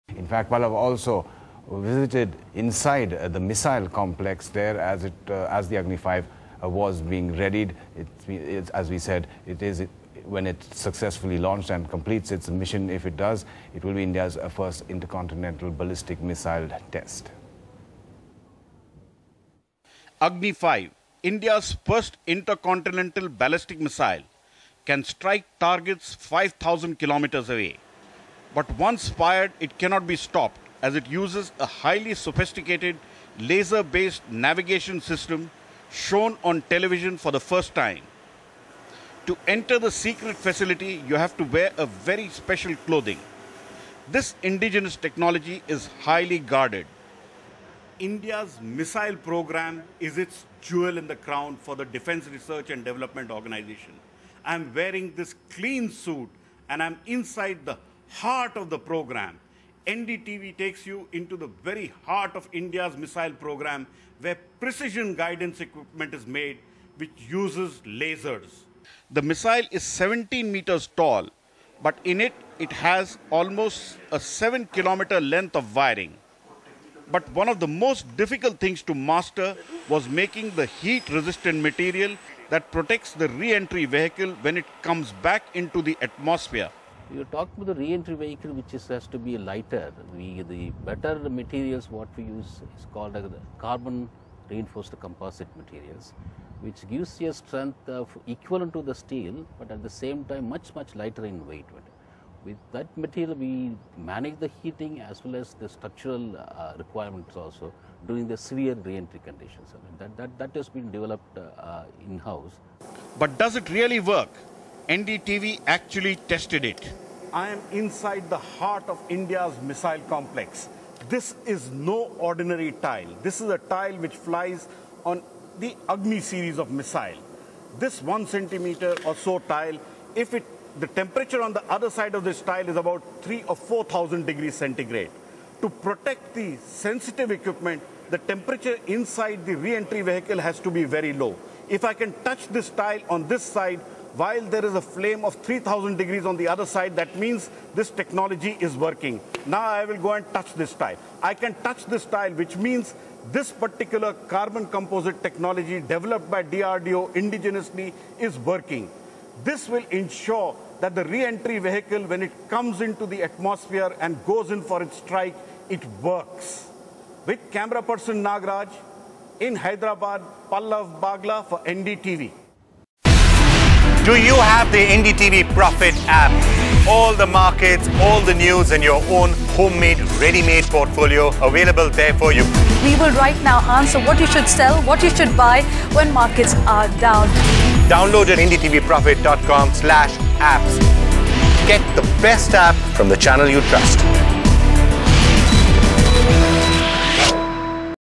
Inside the Agni-5 missile lab
India's first inter-continental ballistic missile. NDTV takes you inside India's missile complex, a highly sophisticated facility in Hyderabad and help you decode the Agni 5.